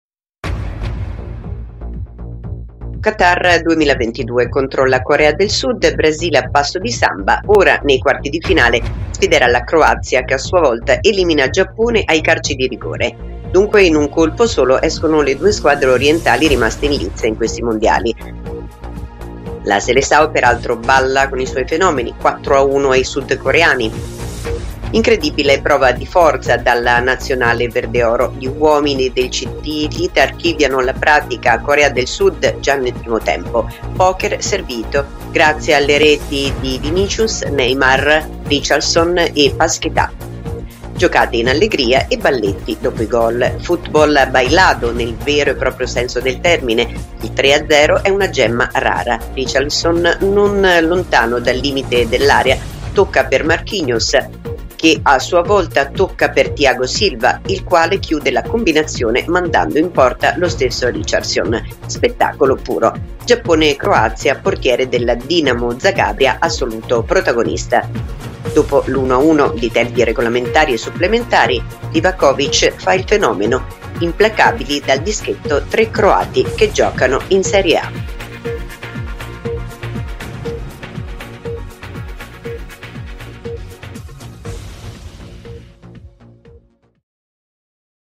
Qatar 2022: troppo Brasile per la Corea. Croazia avanti di rigore
Qatar 2022: contro la Corea del Sud, Brasile a passo di samba; ora nei quarti di finale sfiderà la Croazia che a sua volta elimina il Giappone ai calci di rigore. Dunque, in un colpo solo escono le due squadre orientali rimaste in lizza in questi Mondiali.